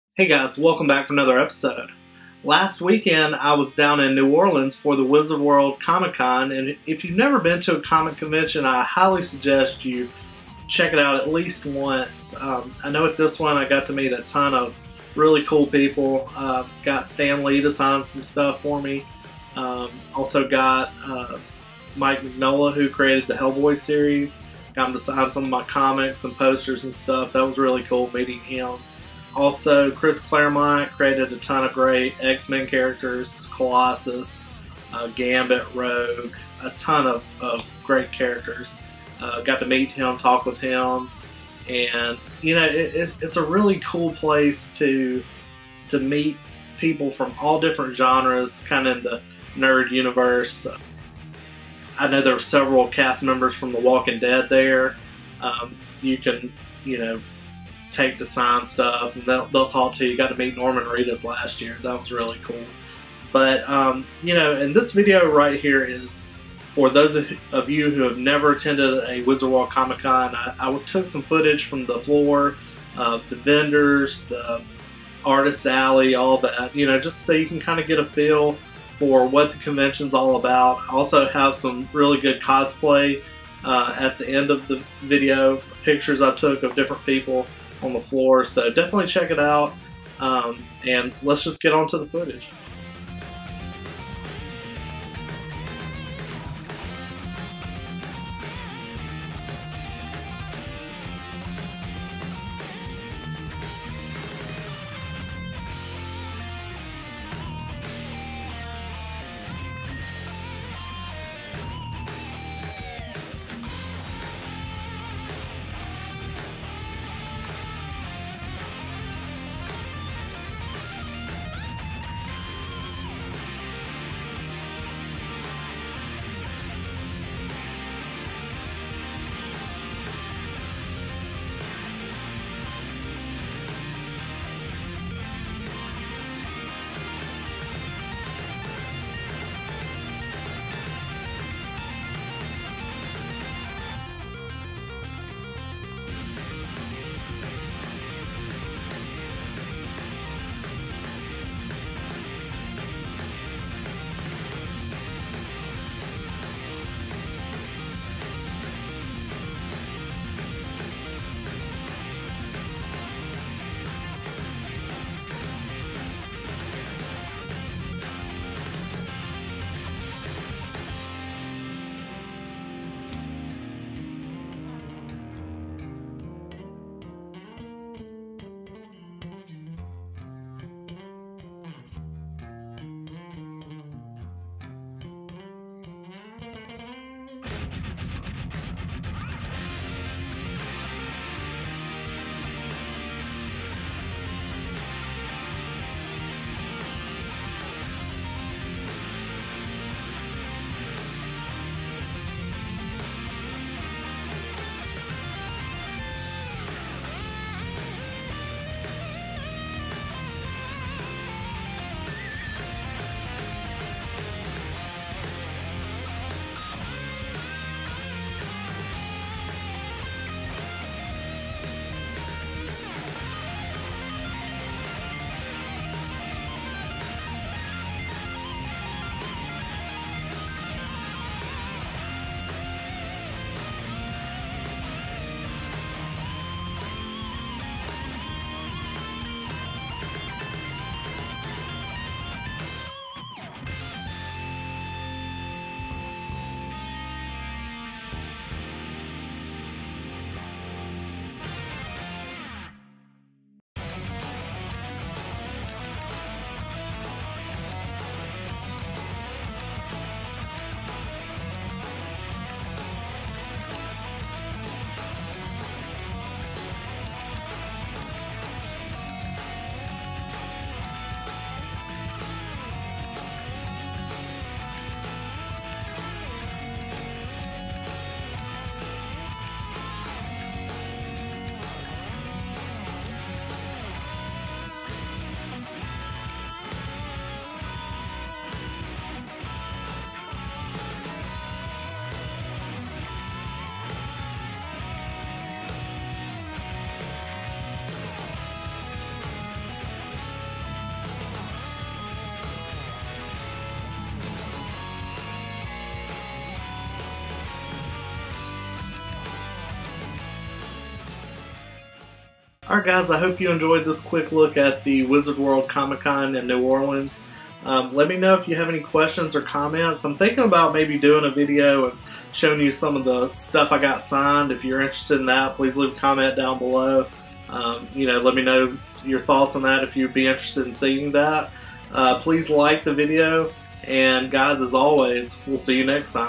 Wizard World Comic Con New Orleans 2014
I took a trip down to the big easy to check out the 2014 comic con